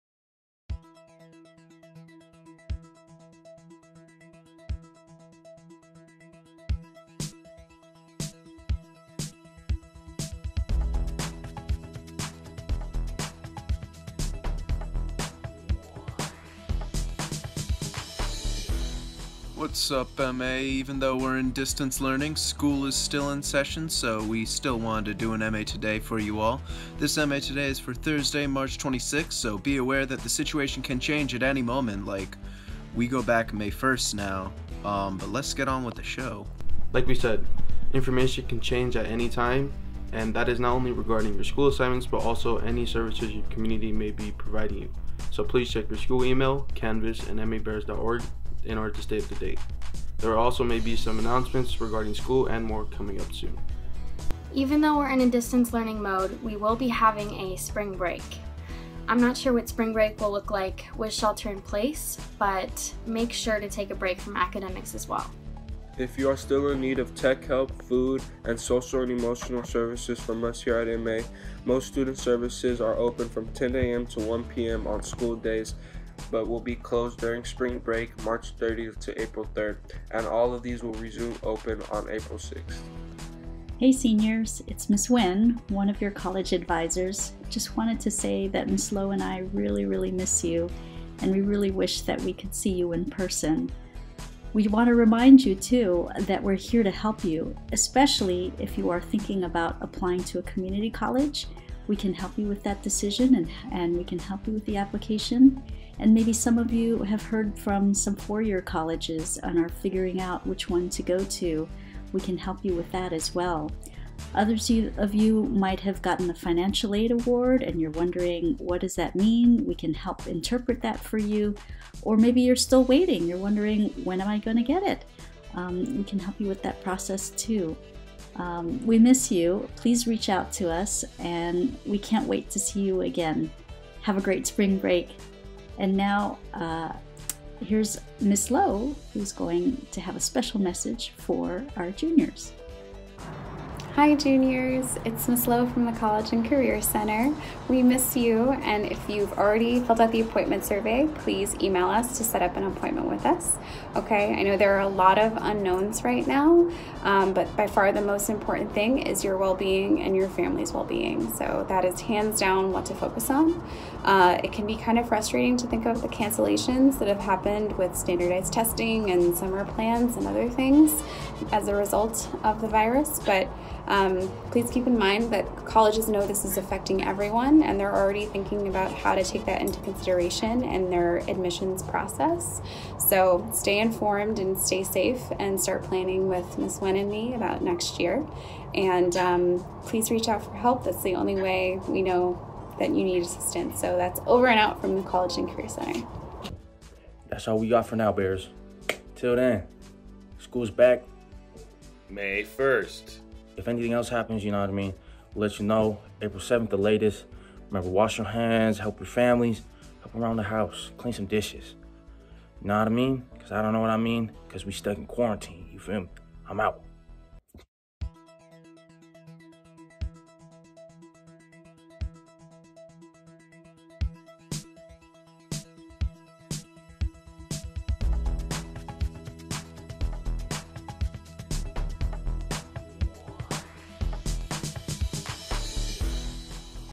M-A Today! for Friday, March 27, Coronavirus Hiatus Week 2
PLEASE CHECK YOUR SEQ EMAIL ACCOUNTS, and CANVAS for school assignments and information about assistance, distance learning, and the school closure.

Campus will be closed Monday, March 30 to Friday, April 3.

Campus will open again, and food and other services will resume, Monday, April 6 from 10 AM -1 PM.

The Collge and Career Center is here to help to:

Ms. Nguyen

Ms. Lowe

For an announcement request, please submit them on our form